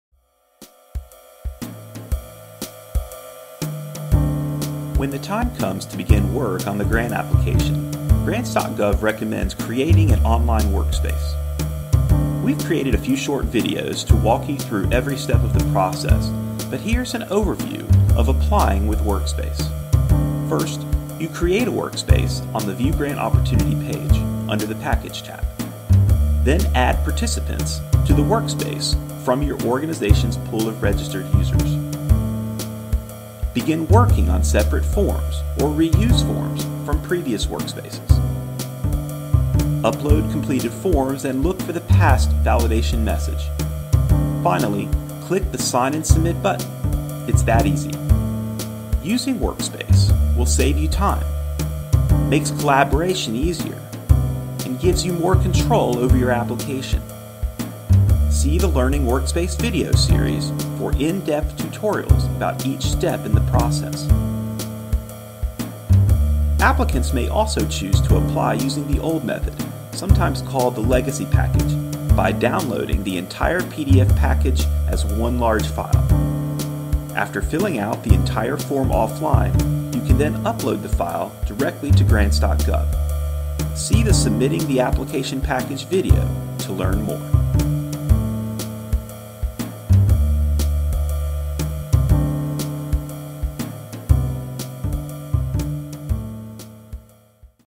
Applying with Workspace on Grants.gov
After prospective applicants have registered with Grants.gov and located a grant opportunity, it's time to apply. This video highlights the benefits of applying with Grants.gov's Workspace feature and points applicants to the Learning Workspace Video Series.

to learn more.